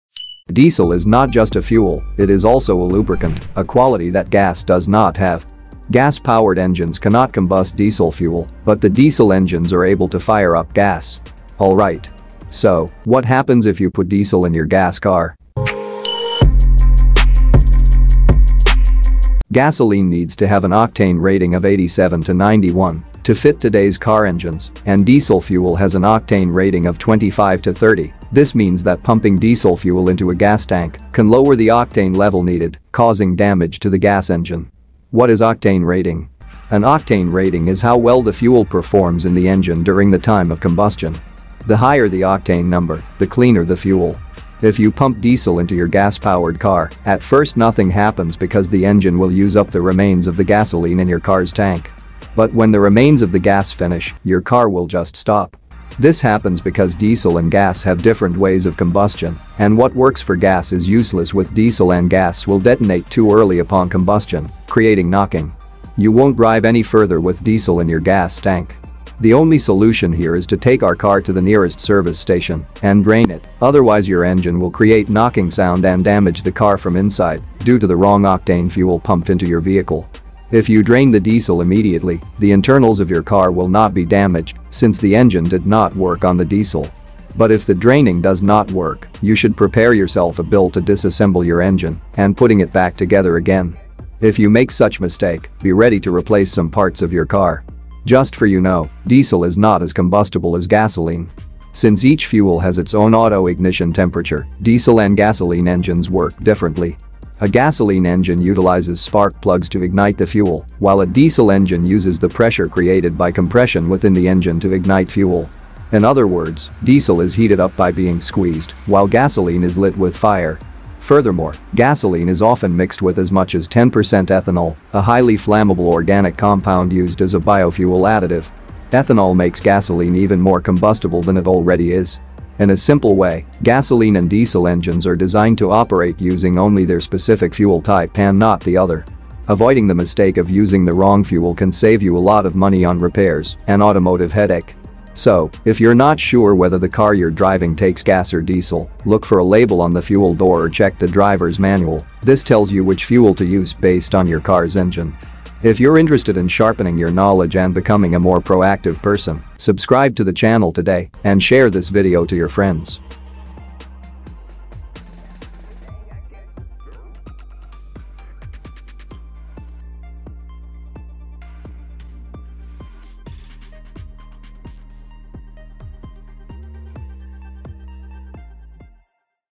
This is WHAT HAPPENS if you put DIESEL in a GAS Car
In this Video, we're going to tell you exactly WHAT HAPPENS if you put DIESEL in a GAS Car. You see, Gasoline needs to have an octane rating of 87 to 91 to fit today's car engines, and Diesel fuel has an octane rating of 25 to 30, this means that pumping diesel fuel into a gas tank can lower the octane level needed, and cause damage to the gas engine.

Diesel is not as combustible as gasoline. Since each fuel has its own auto ignition temperature, diesel and gasoline engines work differently. A gasoline engine utilizes spark plugs to ignite the fuel, while a diesel engine uses the pressure created by compression within the engine to ignite fuel.

Ethanol makes gasoline even more combustible than it already is. In a simple way, gasoline and diesel engines are designed to operate using only their specific fuel type and not the other. So, what happens if you put Diesel in your gas Car? What is octane rating?

Time codes:
0:00-Intro
0:20-The Difference between Diesel and Gasoline
0:35-What is Octane Rating
0:44-What happens if you put Diesel in your Gas Car?
2:25-Diesel or Gasoline
2:35-Subscribe for more

Stay Safe